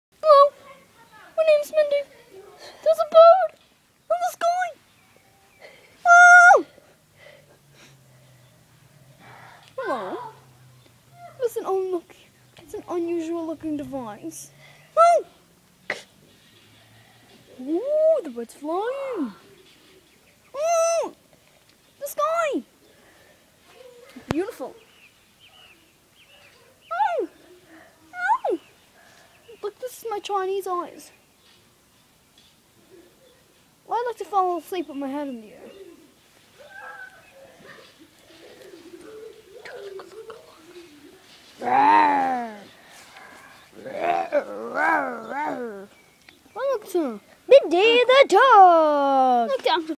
Talking Dog Looks at the sky
lovely Dog Admires the birds in the sky and is just so Crazy :D